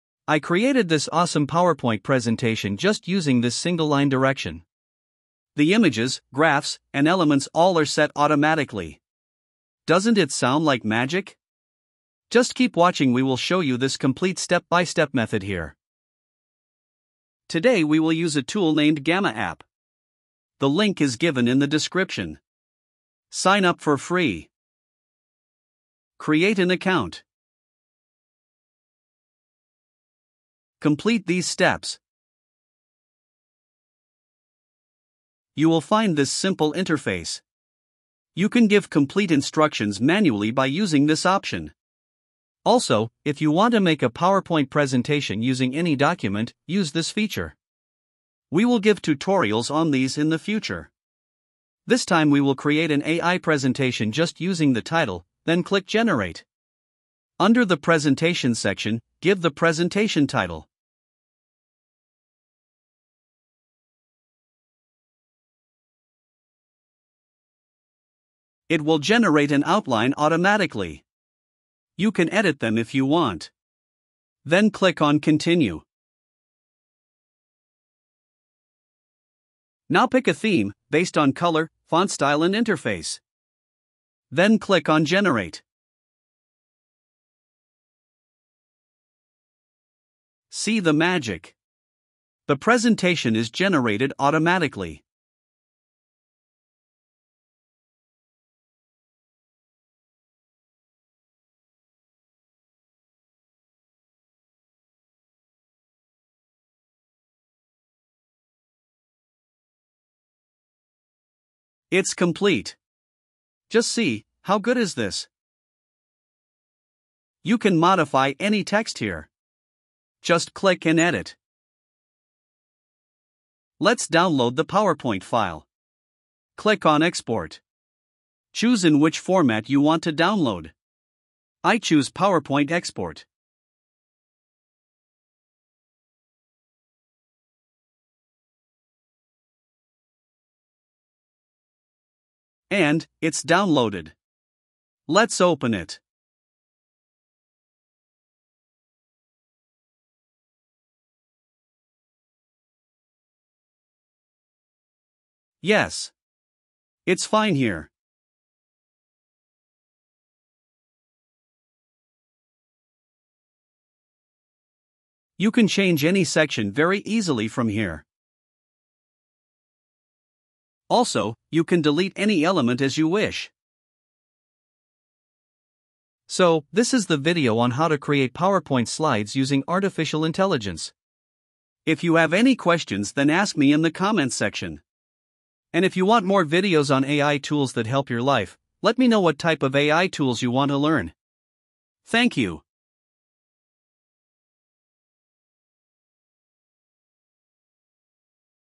How to Make PPT Presentation With Ai for Free | Make Ai PowerPoint Presentation ( Within 2 Minutes )
Learn how to make a fully-designed PowerPoint presentation with AI in just two minutes! In this video, we’ll introduce you to Gamma App, an innovative tool that simplifies presentation creation with a single prompt.

By entering just a title, you can generate a complete PowerPoint, including images, graphs, and layouts—all tailored to your chosen theme. Gamma App handles the design process automatically, saving you time and effort. We’ll guide you through every step, from signing up to downloading your finished presentation as a PowerPoint file, and show you how to make easy edits for any adjustments.

This tutorial is perfect for students, professionals, or anyone wanting to streamline their presentation workflow. Give Gamma App a try and experience the magic of AI in presentation design!

⏩ 2 Years Unlimited Hosting @ $68 Only !!! (Globally Famous Provider)

(Cheapest on the planet 😱😱😱 )